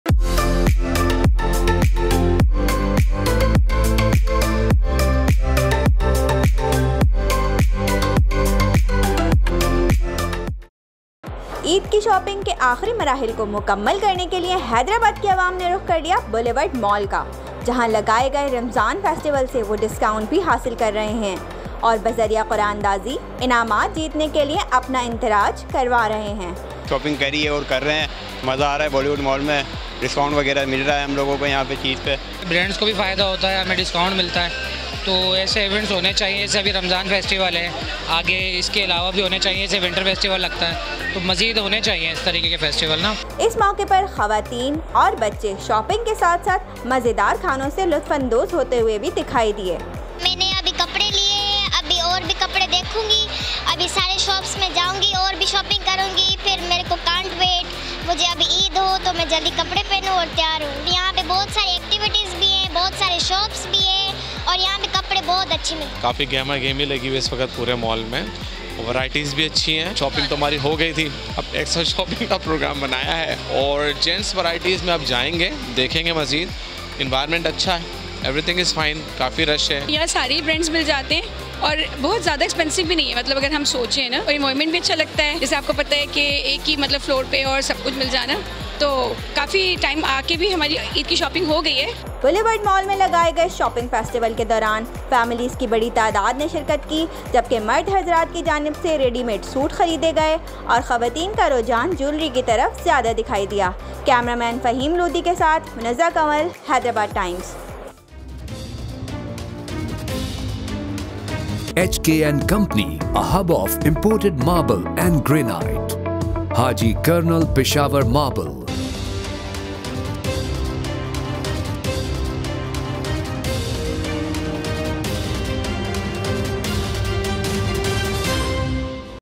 On the occasion of Chand Raat, the people of Hyder responded to Boulevard MallWhere they are
getting chance with shopping rewards.

چاند رات کے موقعے پر حیدرآباد کی عوام نے رخ کرلیا بولیورڈ مال کا
جہاں انہیں خریداری کے ساتھ انعامات جیتنے کا موقع مل رہا ہے